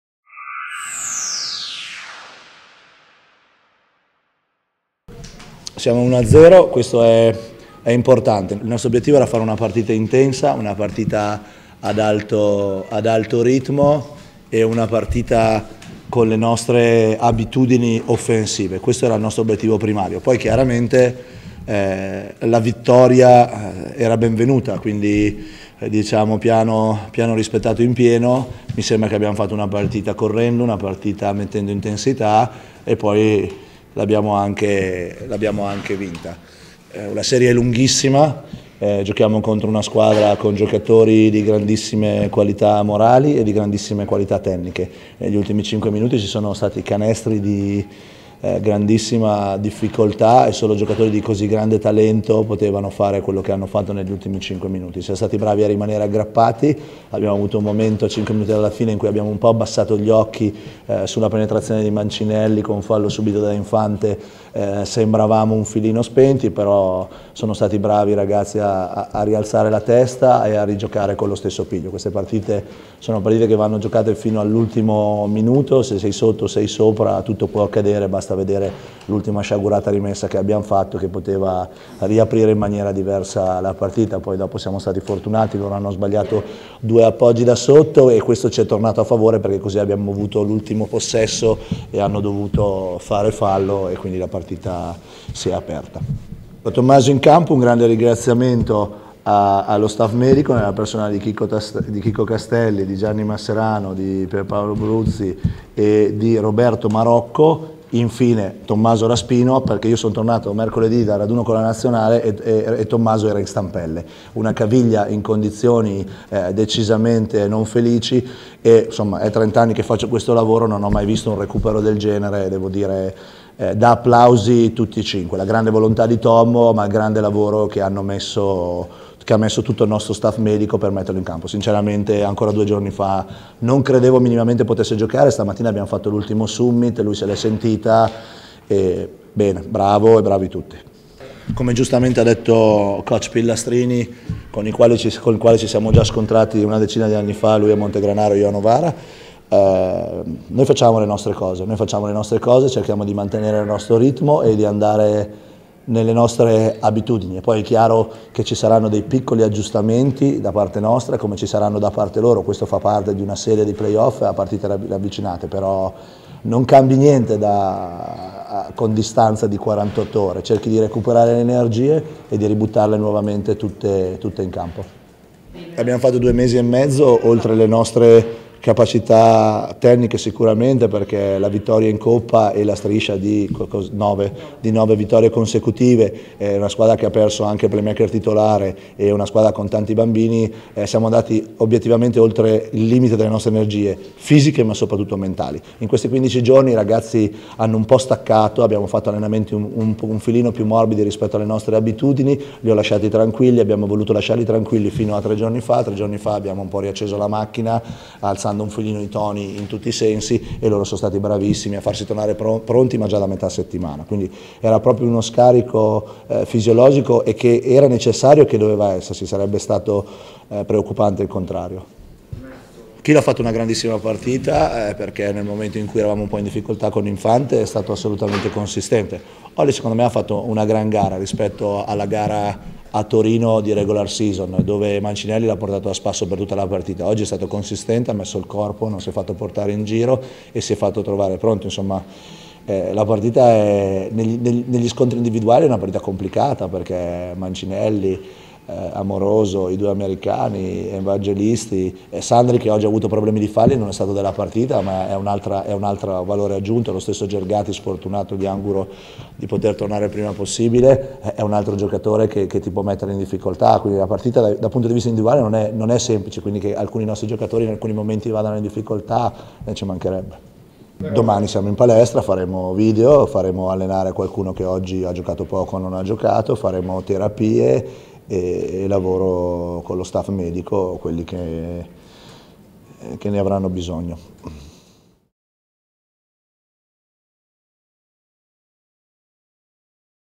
Biella - Torino (Quarti di finale playoff, Gara 1): coach Fabio Corbani in sala stampa
Le dichiarazioni di coach Fabio Corbani in sala stampa nel post-partita di Angelico Biella - Manital Torino, Gara 1 dei Quarti di Finale playoff.
Biella Forum, 4 maggio 2014.